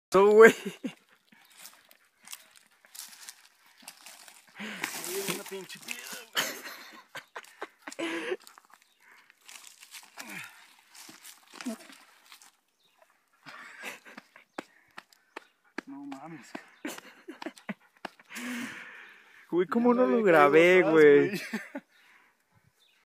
POLLO COMIENDO TIERRA EN EL ASERRIN
AZOTE DE POLLO EN EL CERRO EL ASERRIN... MALDITA SEA QUE NO GRABE EL MOMENTO DE LA CAIDA...